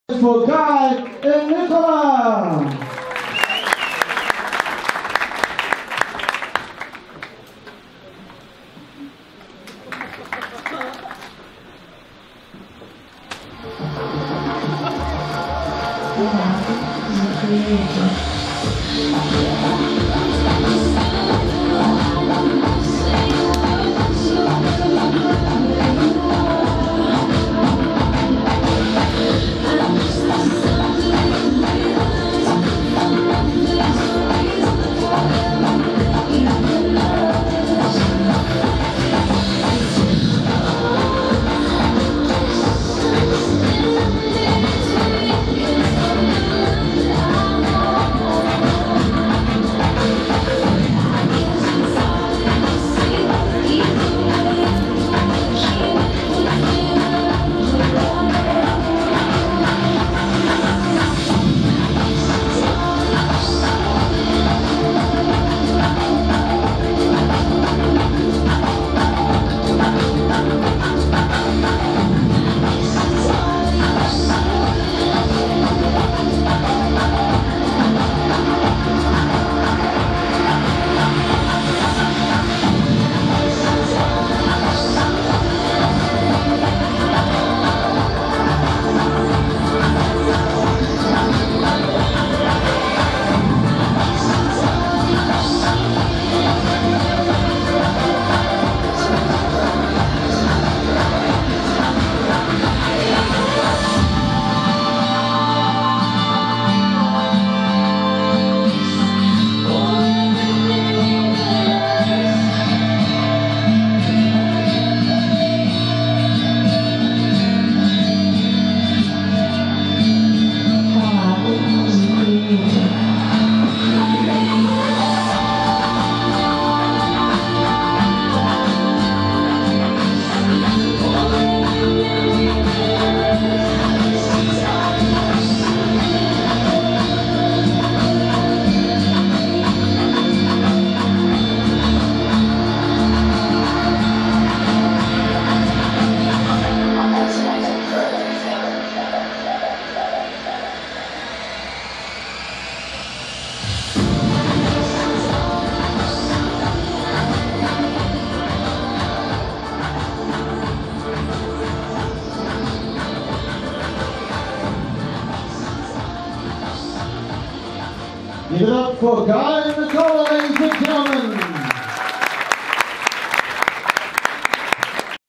Guy & Nicola - Zouk - 4th Premier Freestyle Amateur Latin Dance Championships
4th Premier Freestyle Amateur Latin Dance Championships
Brisbane 2015

This is a freestyle competition format, competitors are performing  to music randomly selected by the DJ.